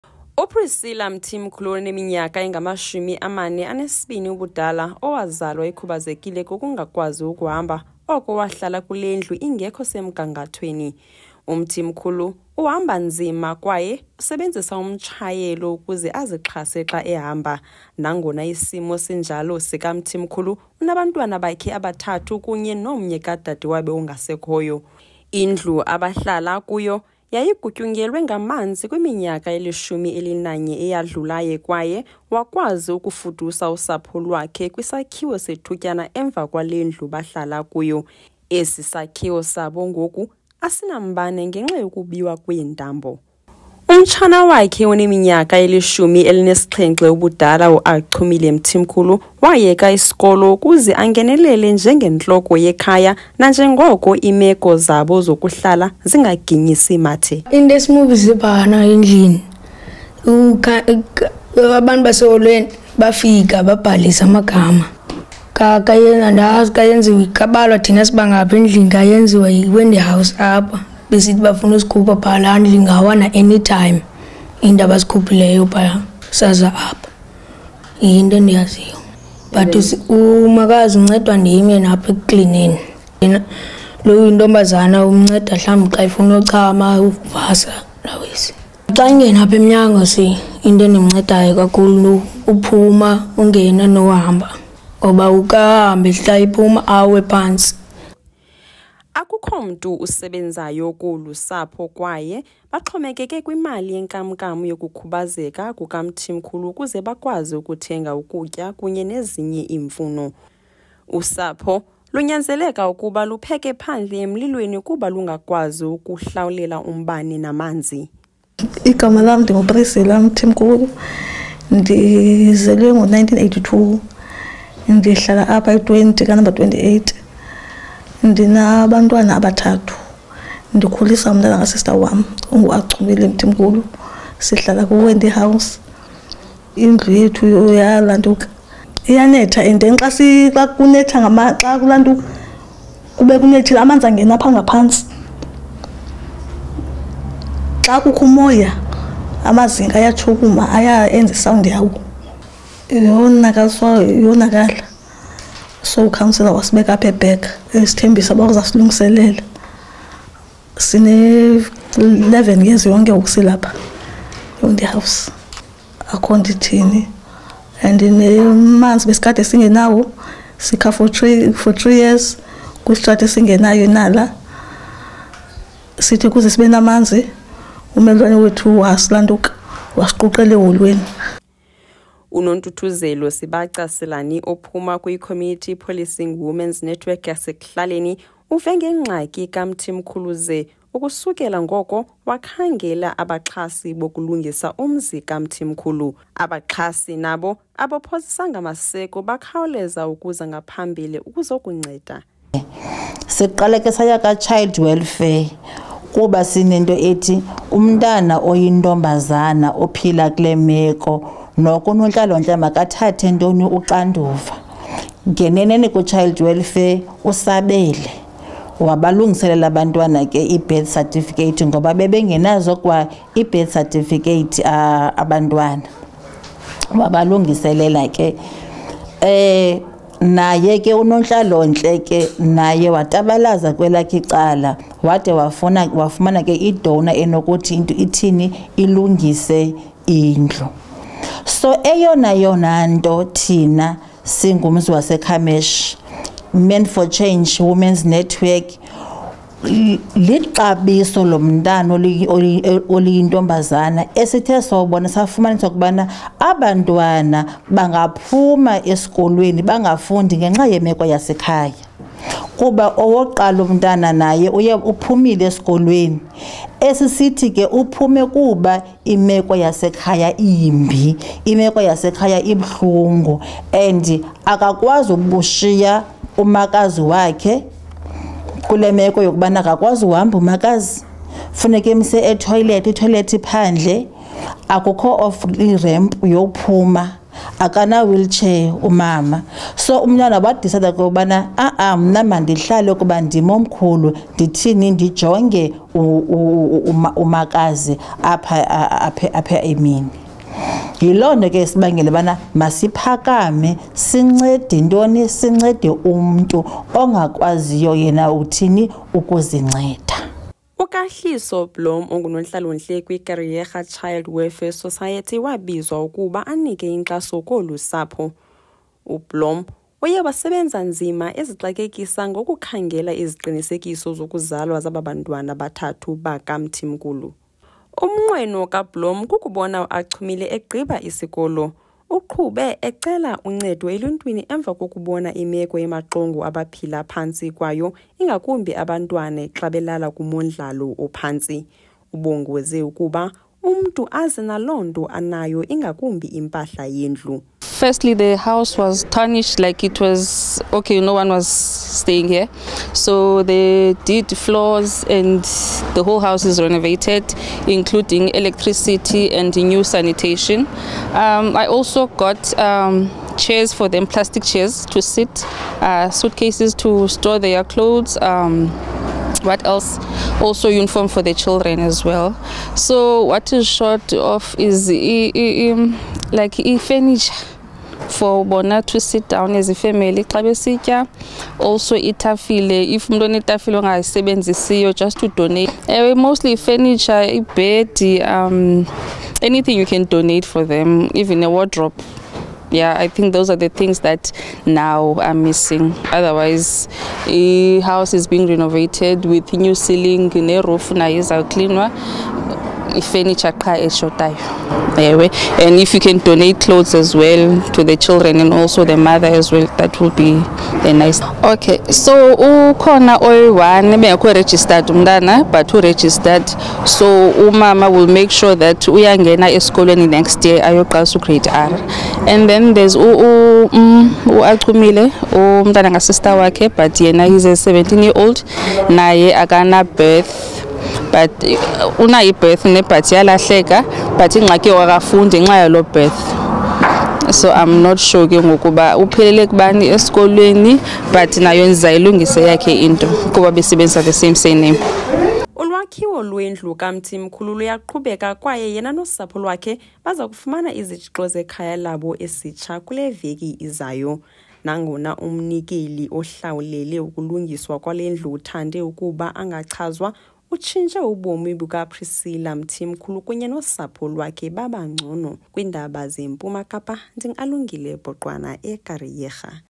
KARIEGA PKG 6PM 180924
Iqonga lamapolisa oluntu laseKariega kunye noluntu lwaKwaNobuhle, basebenze ngokuzimisela bencedisa ekwakhiweni ikhaya elitsha lowasethyini okhubazekileyo. Umsamariya olungileyo nongafunanga kuchazwa igama, uye wahambela phambili ekwakheni lendlu.